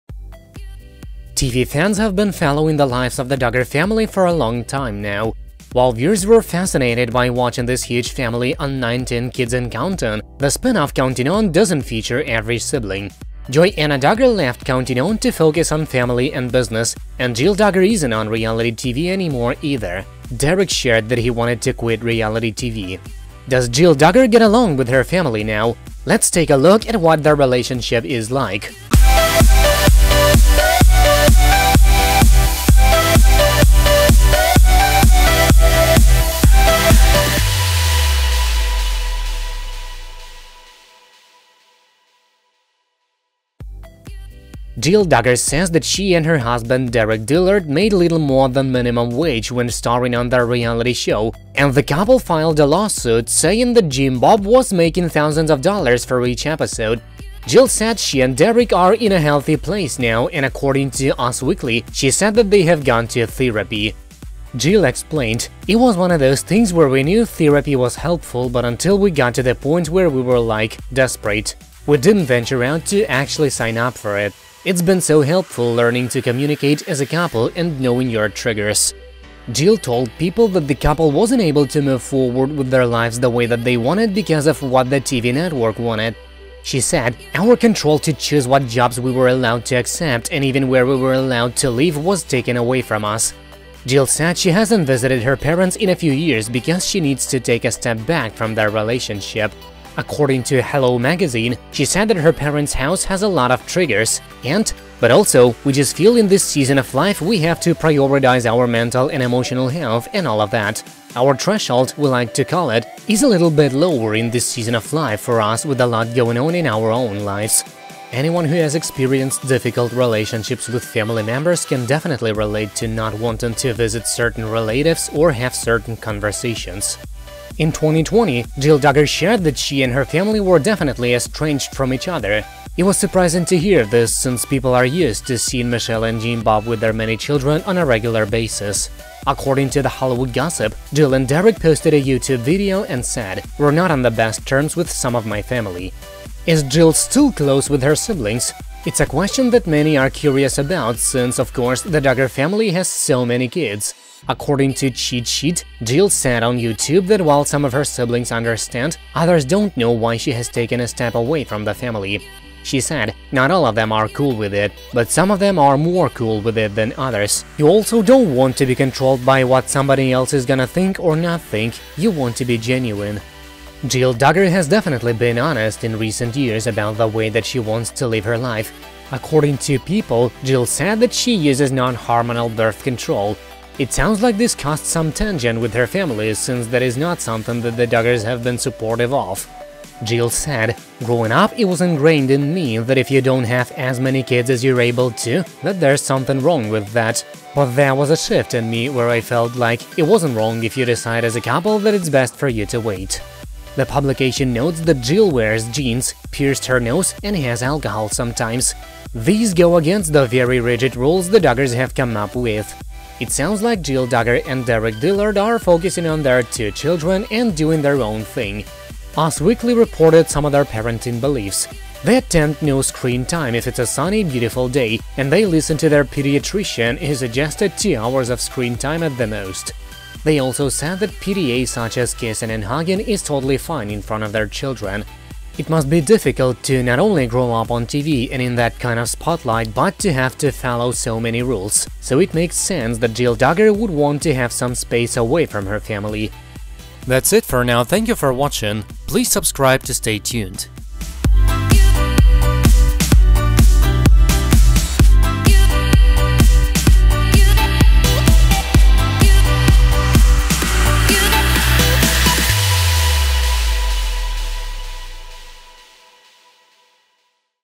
Does Jill Duggar Get Along With Her Family Now? - Counting On TLC show
TV fans have been following the lives of the Duggar family for a long time now. While viewers were fascinated by watching this huge family on 19 Kids And Counting, the spin-off Counting On doesn't feature every sibling.

Joy-Anna Duggar left Counting On to focus on family and business, and Jill Duggar isn't on reality TV anymore, either. Derick shared that he wanted to quit reality TV.

Does Jill Duggar get along with her family now? Let's take a look at what their relationship is like.

The Relationship Now

Jill Duggar says that she and her husband Derick Dillard made a little more than minimum wage when starring on their reality show, and the couple filed a lawsuit saying that Jim Bob was making thousands of dollars for each episode.

Jill said she and Derick are "in a healthy place now" and according to Us Weekly, she said that they have gone to therapy.

Jill explained, “It was one of those things where we knew therapy was helpful but until we got to the point where we were, like, desperate … we didn’t venture out to actually sign up for it. It’s been so helpful learning to communicate as a couple and knowing your triggers.”

Jill told People that the couple wasn't able to move forward with their lives the way that they wanted because of what the TV network wanted. She said, "Our control to choose what jobs we were allowed to accept and even where we were allowed to live was taken away from us."

Jill said she hasn't visited her parents in a few years because she needs to take a step back from the relationship. According to Hello magazine, she said that her parents' house has "a lot of triggers" and "But also, we just feel in this season of life we have to prioritize our mental and emotional health and all of that. Our threshold — we like to call it — is a little bit lower in this season of life for us with a lot going on in our own lives."

Anyone who has experienced difficult relationships with family members can definitely relate to not wanting to visit certain relatives or have certain conversations.

Jill And Her Siblings

In 2020, Jill Duggar shared that she and her family were definitely estranged from each other. It was surprising to hear this since people are used to seeing Michelle and Jim-Bob with their many children on a regular basis.

According to The Hollywood Gossip, Jill and Derick posted a YouTube video and said, "We’re not on the best terms with some of my family."

Is Jill still close with her siblings? It's a question that many are curious about since, of course, the Duggar family has so many kids.

According to Cheat Sheet, Jill said on YouTube that while some of her siblings understand, others don't know why she has taken a step away from the family. She said, “Not all of them are cool with it. But some of them are more cool with it than others. … You also don’t want to be controlled by what somebody else is gonna think or not think. You want to be genuine.”

Finding Her Own Journey

Jill Duggar has definitely been honest in recent years about the way that she wants to live her life. According to People, Jill said that she uses non-hormonal birth control. It sounds like this caused some tension with her family since that is not something that the Duggars have been supportive of.

Jill said, "Growing up, it was ingrained in me that if you don't have as many kids as you're able to, that there's something wrong with that. But there was a shift in me where I felt like it wasn't wrong if you decide as a couple that it's best for you to wait."

The publication notes that Jill wears jeans, pierced her nose, and has alcohol sometimes. These go against the very rigid rules the Duggars have come up with.

It sounds like Jill Duggar and Derick Dillard are focusing on their two children and doing their own thing. Us Weekly reported some of their parenting beliefs. They attempt no screen time if it's a sunny, beautiful day, and they listen to their pediatrician who suggested two hours of screen time at the most. They also said that PDA such as kissing and hugging is totally fine in front of their children.

It must be difficult to not only grow up on TV and in that kind of spotlight but to have to follow so many rules, so it makes sense that Jill Duggar would want to have some space away from her family.